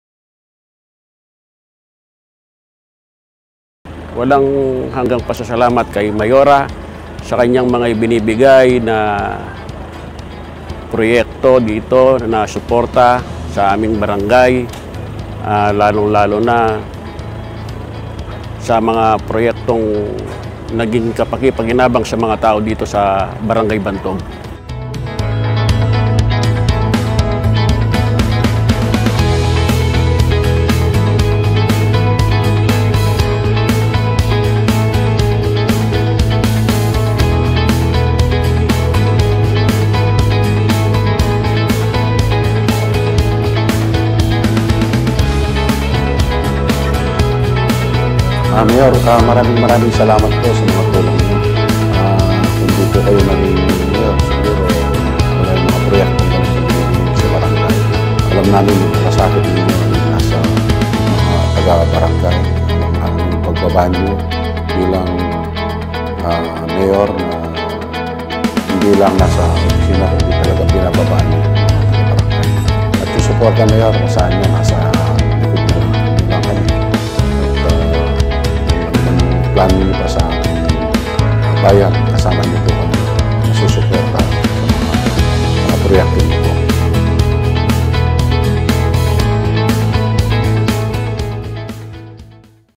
Serbisyong Anghel sa Barangay Bantog
Tunghayan ang mga serbisyo, programa at proyekto ng City Government of Tarlac sa pamumuno ni Mayor Cristy Angeles para sa mga residente ng Barangay Bantog

Tunay na Serbisyong Anghel kung kaya lubos ang pasasalamat ng pamunuan ng barangay at mga residente nito.

Maraming salamat sa iyong pagmamahal, Mayor Cristy!